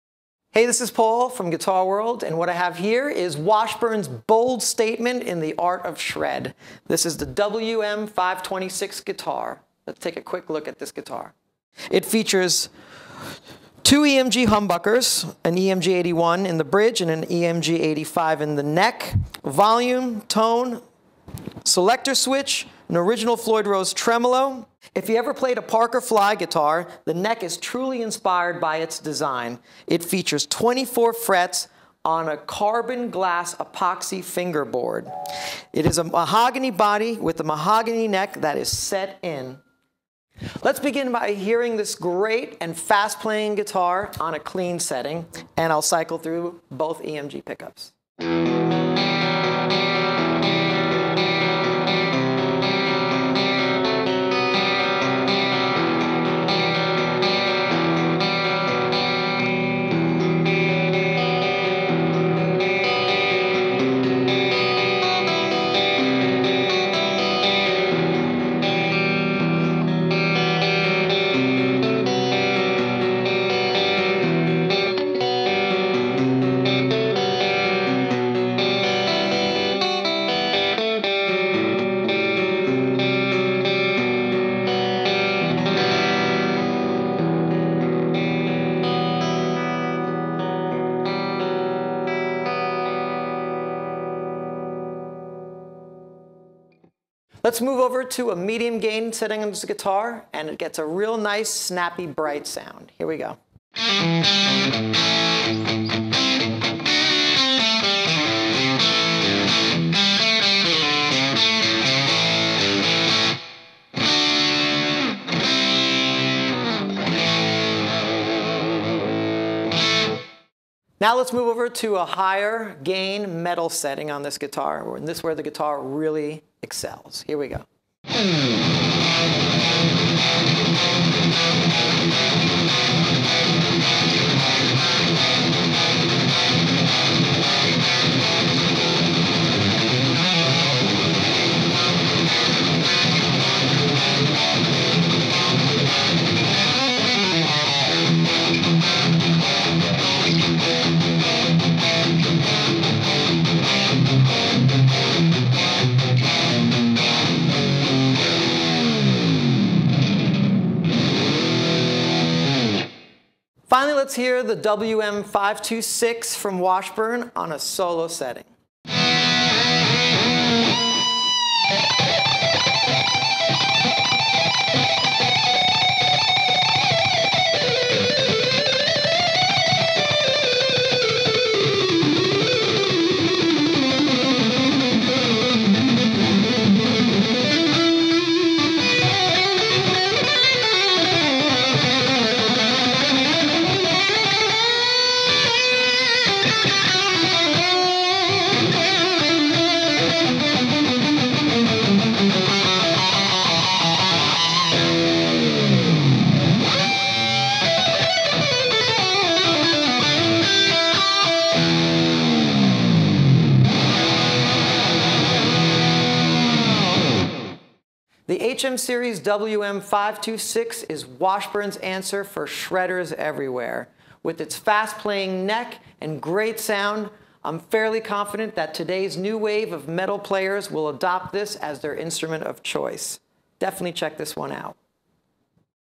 Washburn HM Series WM526 Guitar
For more gear reviews and videos check out guitarworld.com

In this video Guitar World's gear editor Paul Riario demonstrates the features of the Washburn HM Series WM526 guitar.